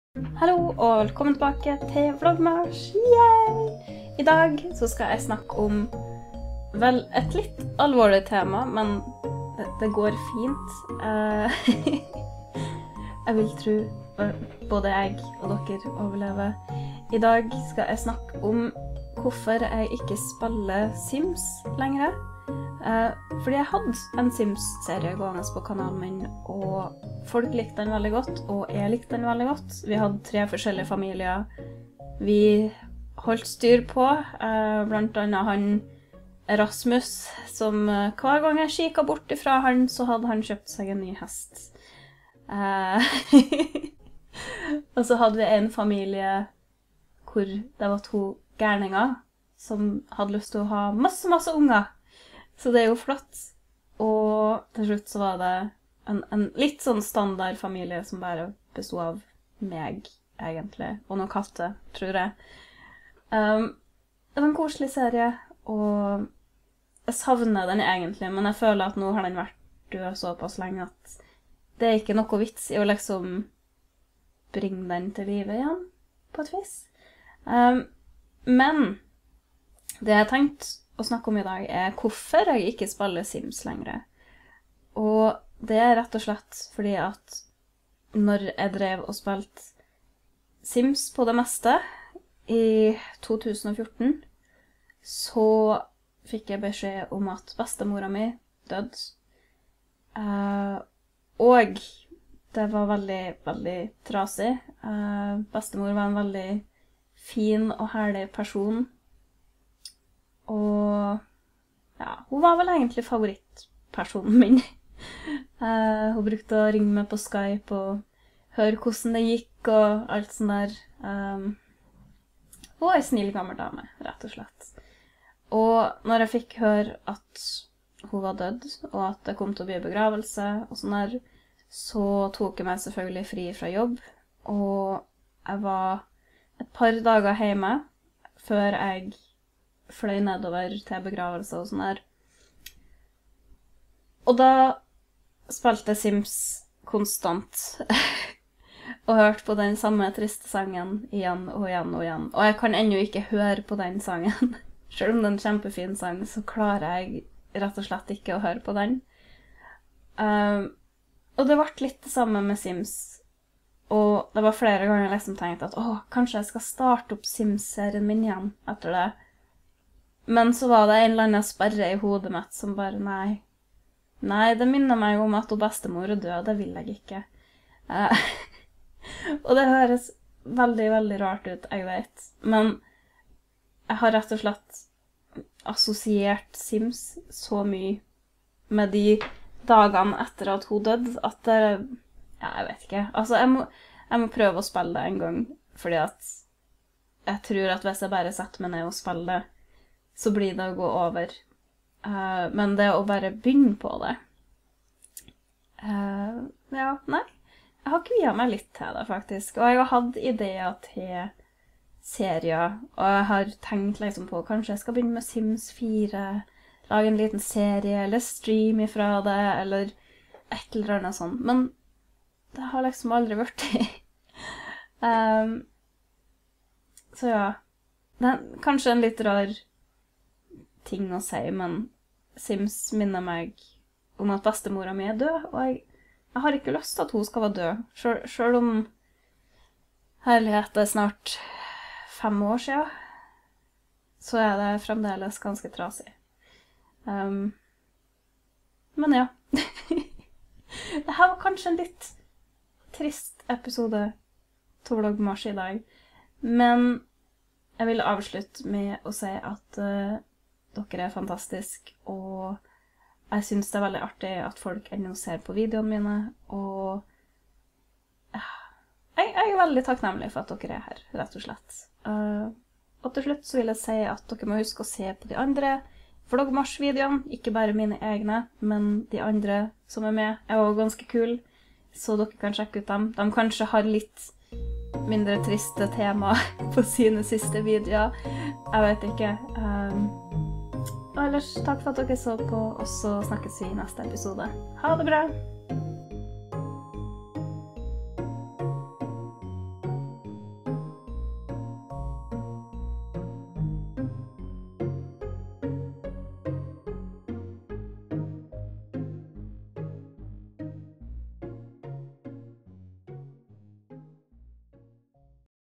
Hvorfor spiller jeg ikke Sims 3 lengre? - VlogMars
A - Tillatt for alle! | En litt trist video i dagens VlogMars, men jeg begynte å tenke på dette en dag og det gikk opp et lys for meg. Hvorfor spiller jeg ikke Sims 3 lengre?

Intro- og outromusikk laget av: Inge Jard Grønås.